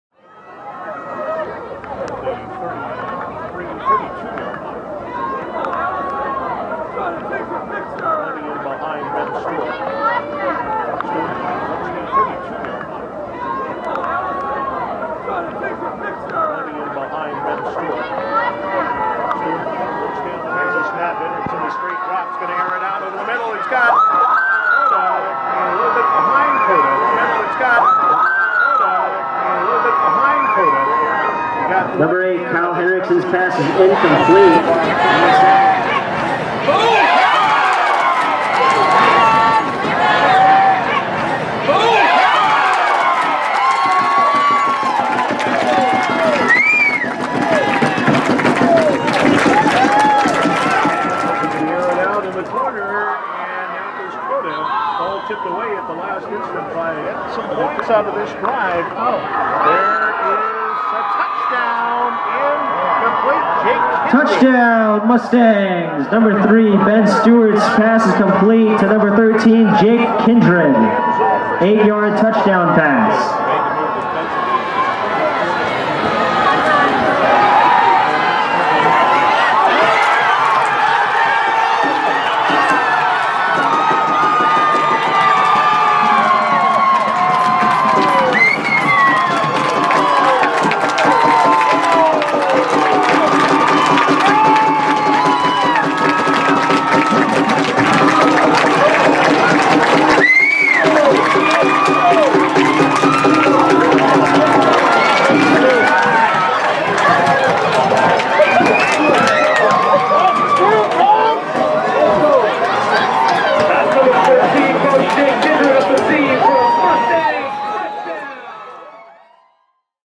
JAKE KINDRED!!! ~ SPECIAL K!!! ~ MU-TANG FOREVER!!!
A Star Is Rising in Chugiak!! Baseball Stand Out since his Freshman Year, Jake Kindred Is A Rising Star in the Highly Ranked Chugiak Mustang Football Program!!! Impact!! Clever Receiver on O, Stick OnYa Defender!! Jake Kindred and the Chugiak Mustangs have a Good Think Going!! Alaska high school football video highlights.